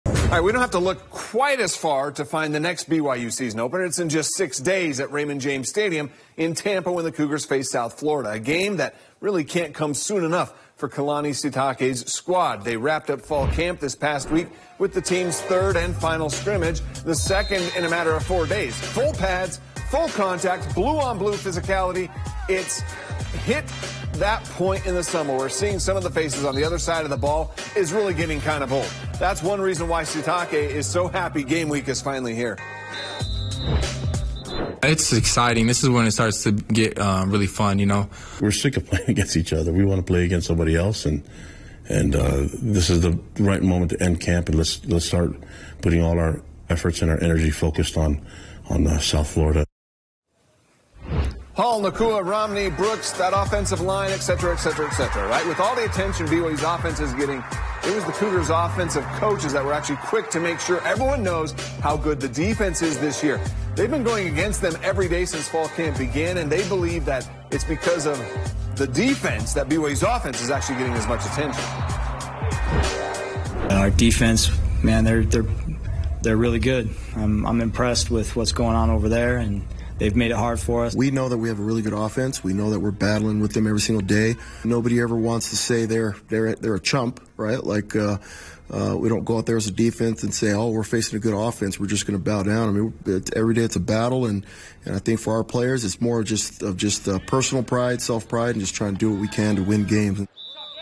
BYU Football Is Excited To Face Another Team Following Long Fall Camp
Fall camp is a month-long grind and the BYU Cougars are finally ready to face another team rather than their teammates.